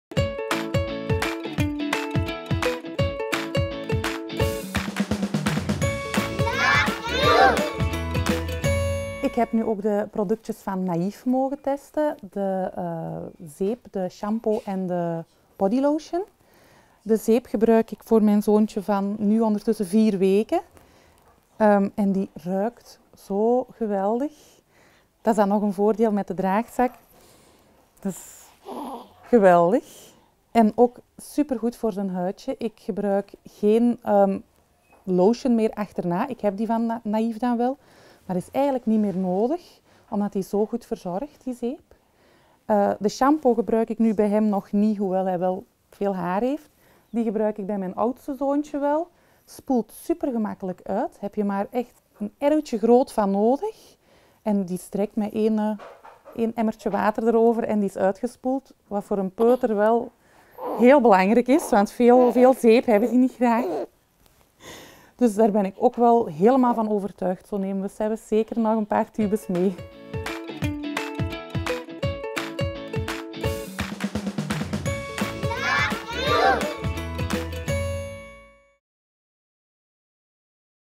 Inne over de verzorgingsproducten van Naïf
Inne, klant van Blabloom, vertelt over de verzorgingsproducten van Naïf.